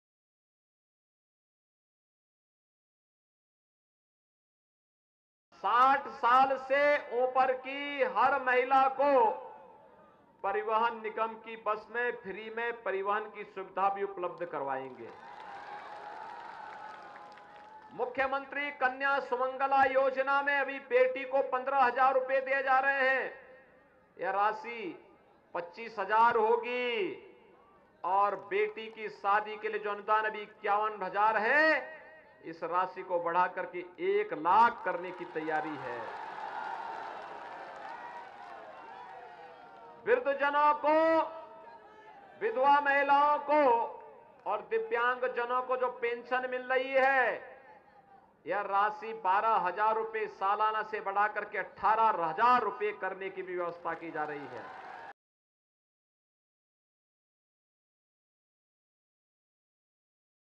UP Polls: CM Yogi promises to increase benefit under Kanya Sumangla Yojana to Rs 25,000
Bahraich (Uttar Pradesh), Feb 24 (ANI): Amid Uttar Pradesh Assembly Elections, Chief Minister Yogi Adityanath on February 24, while addressing a public rally in Bahraich, Uttar Pradesh made slew of promises to women of the state. “Under Mukhayamantri Kanya Sumangla Yojana, Rs 15,000 is being given to daughters. It will be increased to Rs 25,000 and the amount for their marriages will be increased from Rs 51,000 to Rs 1 lakh,” said Yogi Adityanath. UP Legislative Assembly Elections are being held in seven phases. Bahraich will go to polls on February 27 in the fifth phase. Results will be declared on March 10. (SA)